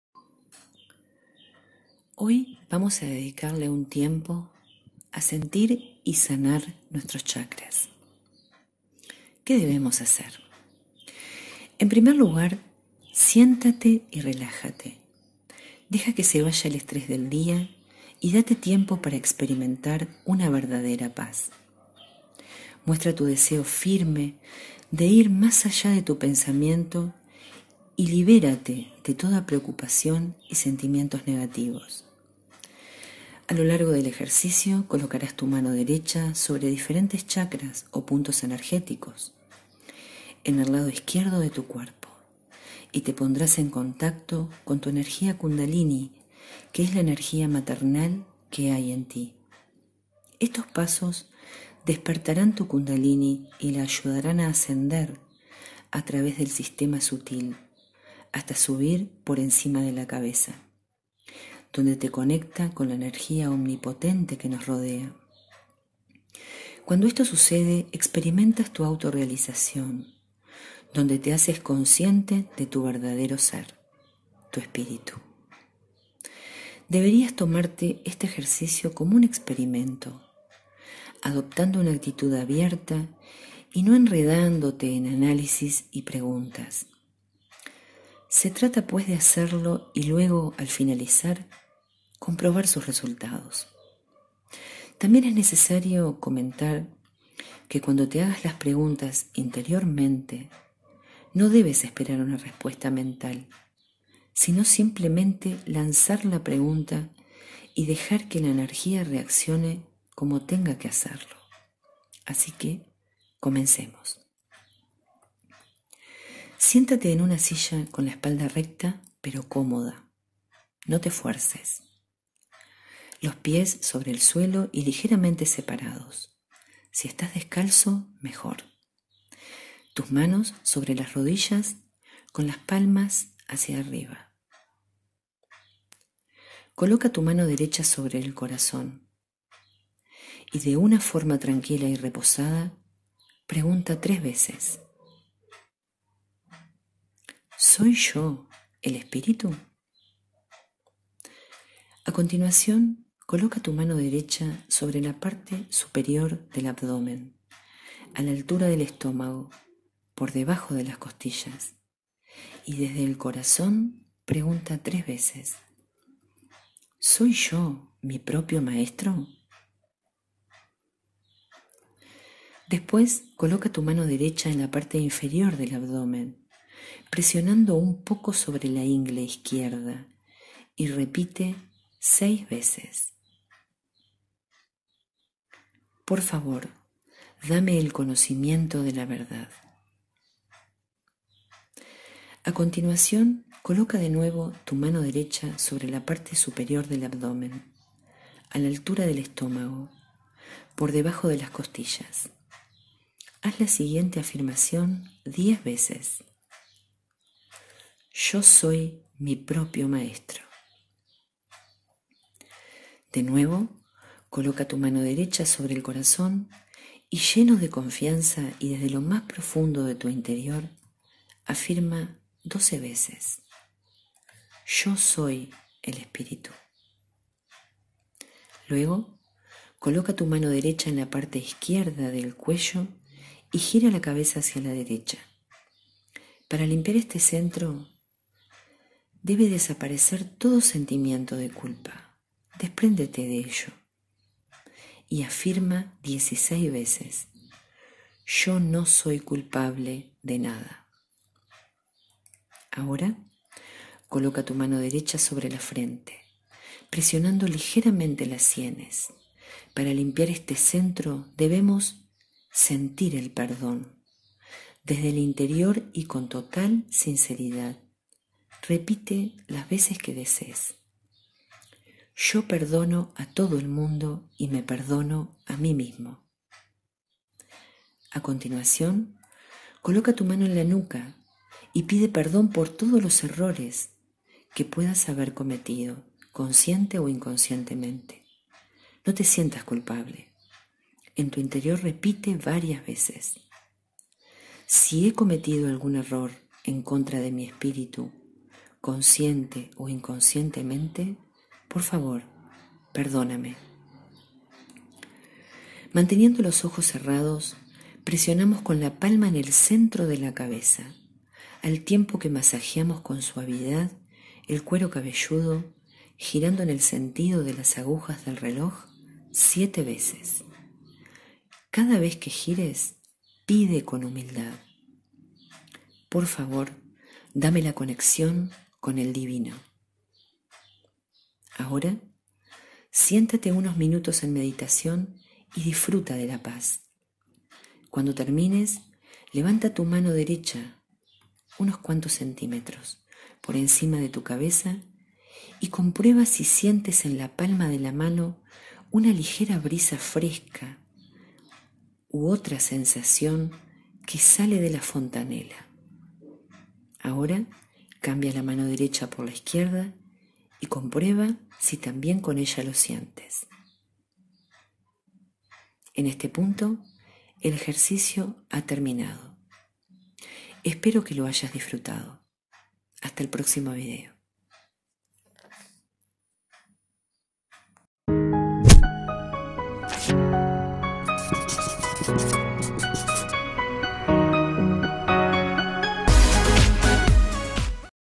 EJERCICIO PARA SENTIR Y LIMPIAR LOS 7 CHAKRAS
Qué hacer❓
En primer lugar, siéntate y relájate. Deja que se vaya el estrés del día y date tiempo para experimentar una verdadera paz. Muestra tu deseo firme de ir más allá de tu pensamiento y libérate de toda preocupación y sentimientos negativos.
A lo largo del ejercicio, colocarás tu mano derecha sobre diferentes chacras o centros de energía en el lado izquierdo de tu cuerpo, y te pondrás en contacto con tu energía Kundalini, que es la energía maternal que hay en ti. Estos pasos despetarán tu Kundalini y la ayudarán a ascender a través del sistema sutil, hasta subir por encima de la cabeza, donde te conecta con la energía omnipresente que nos rodea. Cuando esto sucede, experimentas tu autorrealización, donde te haces consciente de tu verdadero ser: el Espíritu.
Deberías tomarte este ejercicio como un experimento, adoptando una actitud abierta y no enredándote en análisis y preguntas. Se trata, pues, de hacerlo y luego, al finalizar, comprobar sus resultados. También es necesario comentar que cuando te hagas las preguntas interiormente no hay que esperar una respuesta mental, sino simplemente lanzar la pregunta y dejar que la energía reaccione como tenga que hacerlo.

Comenzamos...!!